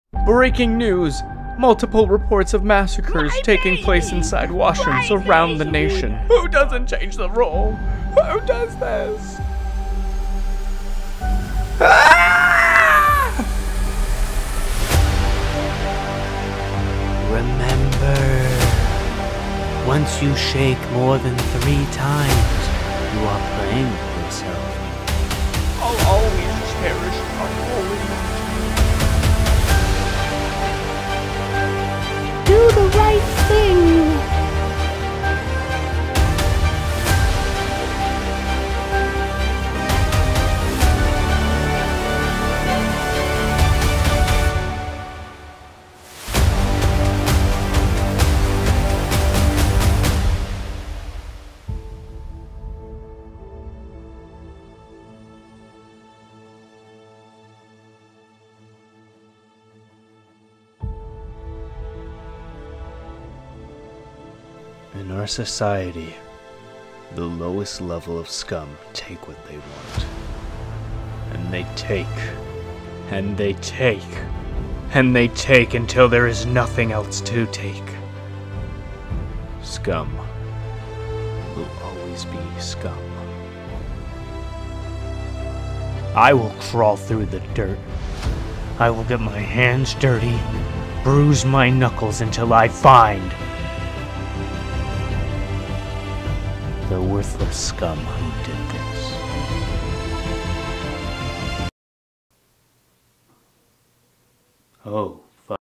The Last Wipe - [Parody Trailer]
This halloween season, ghouls and witches aren't the only things you need to be afraid of...
Beware the man who leaves the roll of toilet paper empty the next time you sit upon your porcelain thrown.